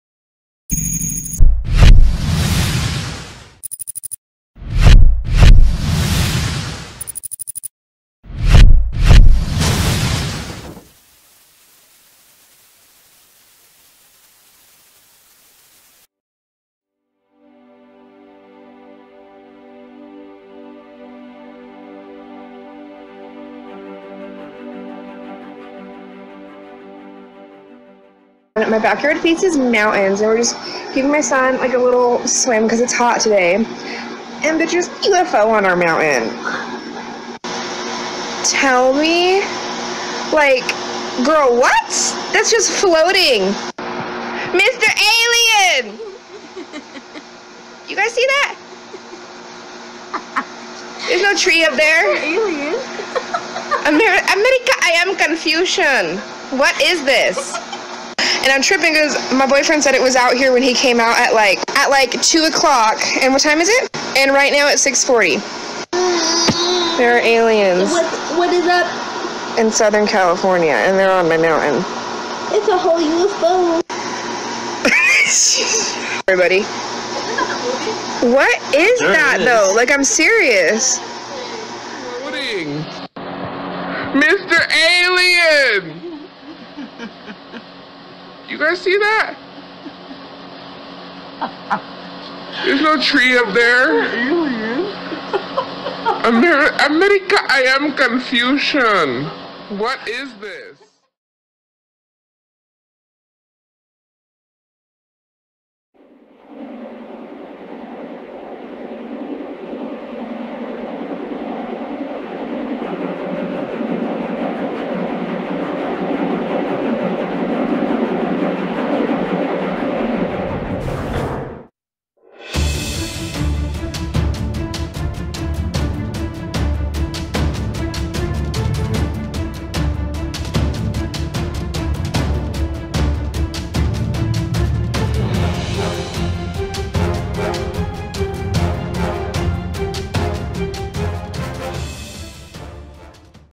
OVNI EN INDIO, CALIFORNIA (21 de Septiembre 2020) / UFO SPOTTED IN INDIO CALIFORNIA (Sept 21, 2020)
El pasado 21 de Septiembre de 2020 , en Indio California en Estados Unidos, una usuaria de "Tik Tok" mientras transmitía en vivo desde su casa captó lo que parecía ser un "platillo volador" suspendido a pocos metros sobre la cima de un cerro cercano.

On September 21, 2020, a "Tik Tok" user in Indio California (USA) while broadcasting live from her home captured what appeared to be a "flying saucer" suspended a few meters above the top of a nearby hill.

Crédito / Source: TIK TOK: @dropofgold